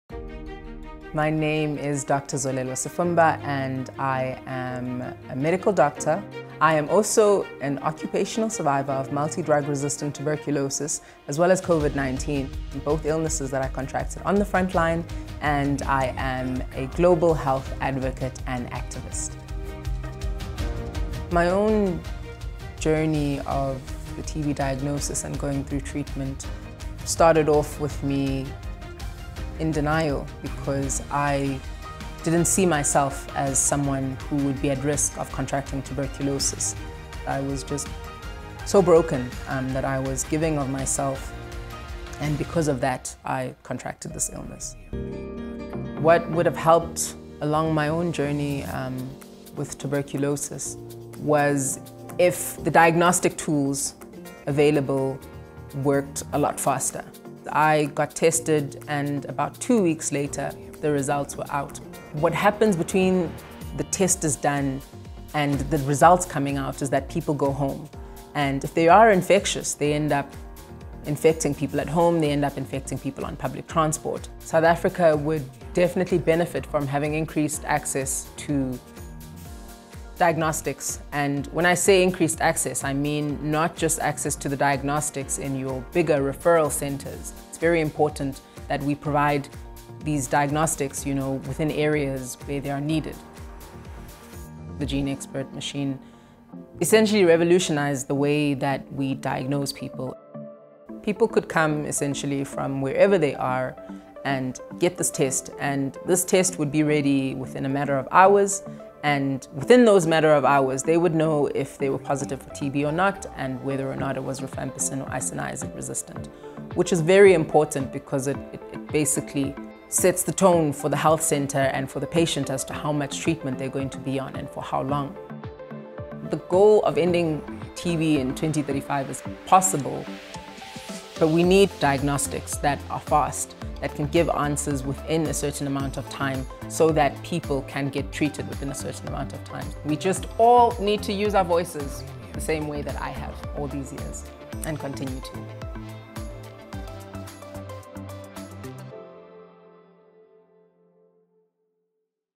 COVID-19 and Tuberculosis Testing in South Africa, A Story from the Doctor and Patient
Dr. Sifumba is a medical doctor, a survivor of both COVID-19 and multidrug-resistant tuberculosis (MDR-TB), and a health activist living in Johannesburg, South Africa. She shares her story and her dedication to improving access to MolecularDiagnostics for patients and health workers across the globe.